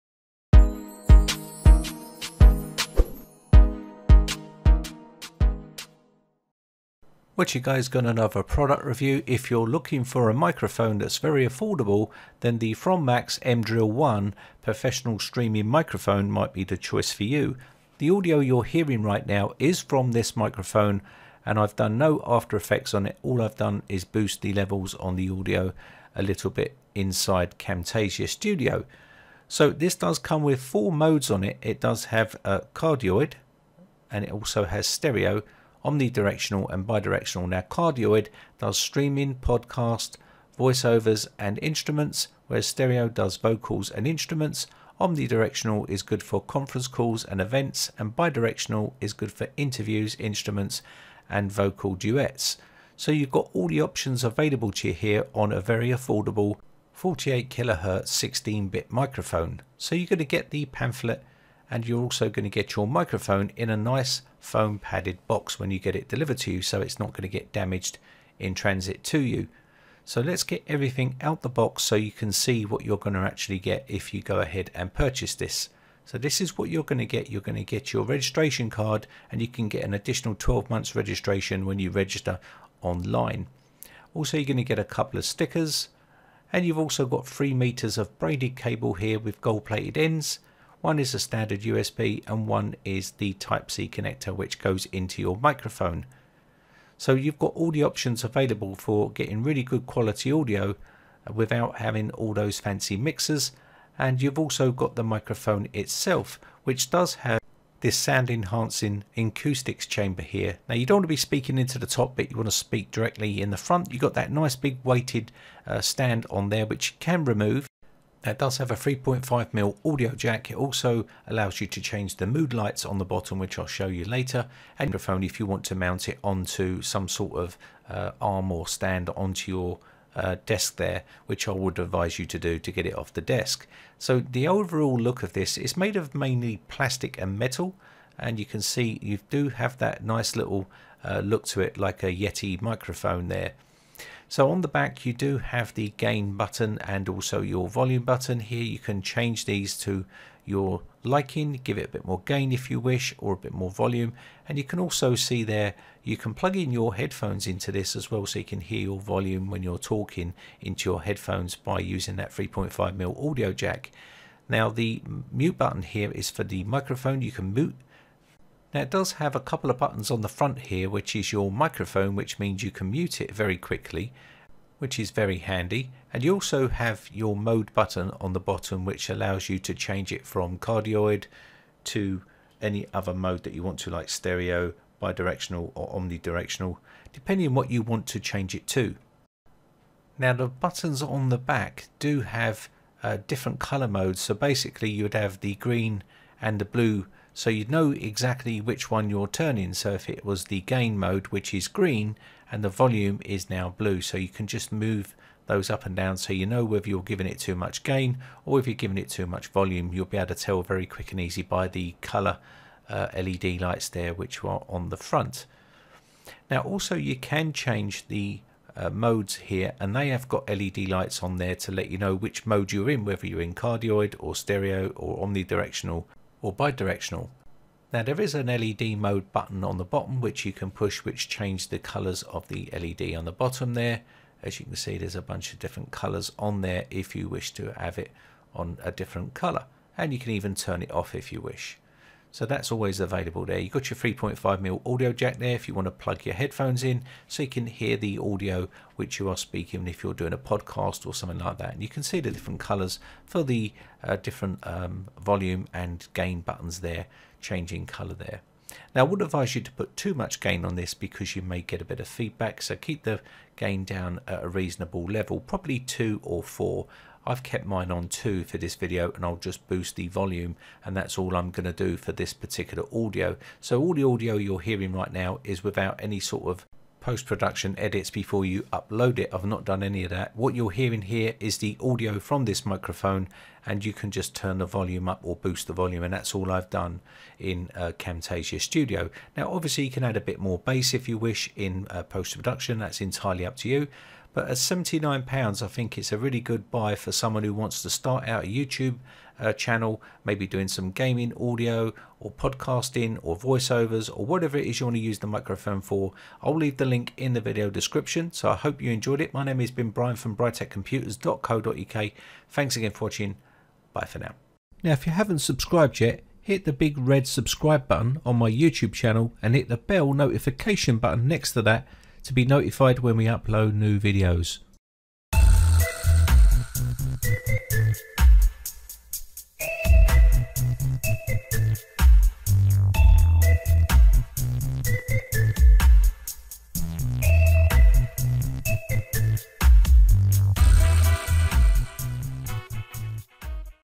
Best Budget Microphone for YouTube
Best Budget Microphone for YouTube 2019

This video covers the best cheap microphone for creating YouTube videos. Looking for a quality microphone for streaming, gaming, podcast, singing and more? well why not try the Thronmax MDrill One Streaming USB Microphone，HD Studio Broadcasting Recording Cardioid Condenser Microphone 48Khz 16Bit, Brand New

Important: the audio you hear in the video is from the Thronmax MDrill One on Cardioid setting. I added some volume and a touch of bass in post production, that's it. Audio was recorded without acoustic foam and you might a slight echo which is my room.